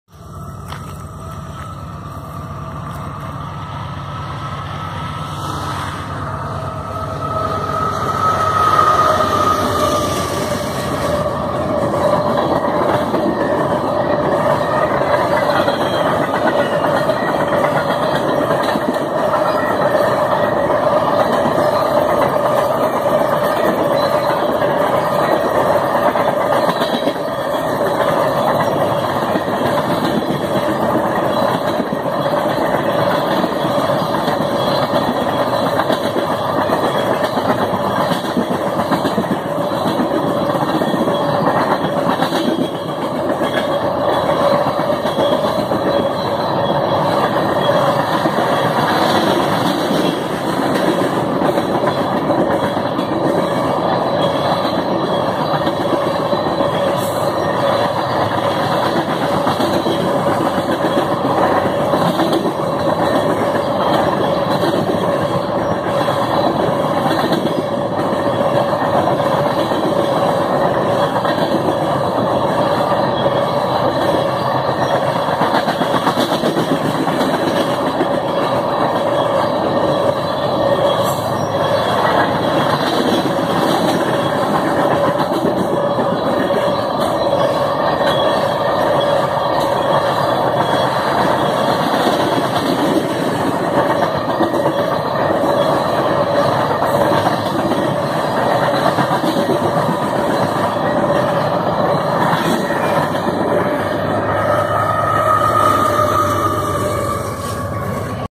CP 575
CP 8635 AC4400
Rear DPU's
KCS 4797 ES44AC
CP 8142 AC4400CWM
WB/Main Track/MP 52.9/Paynesville sub/Time through @7:13 p.m.